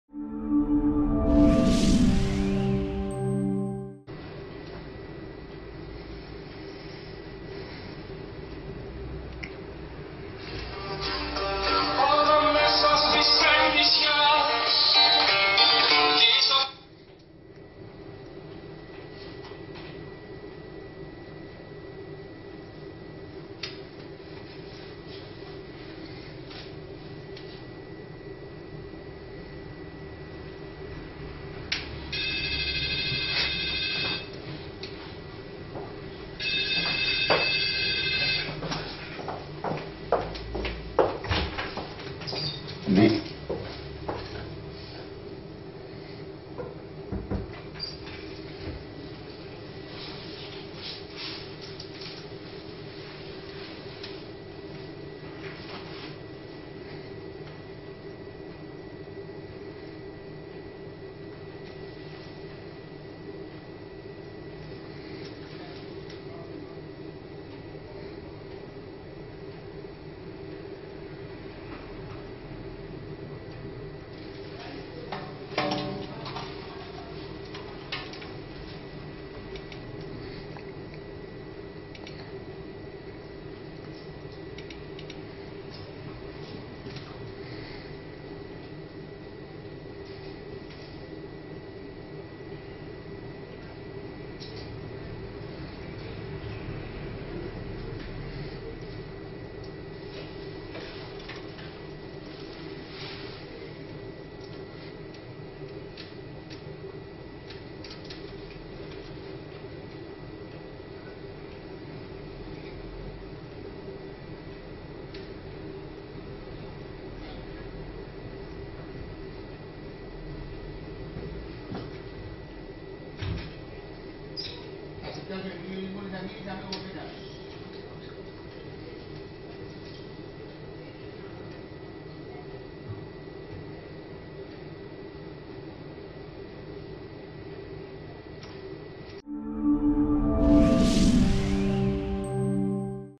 Πάφος : Καρέ - καρέ η δράση του ζεύγους που άρπαξε τα δακτυλίδια
Τι κατέγραψαν οι κάμερες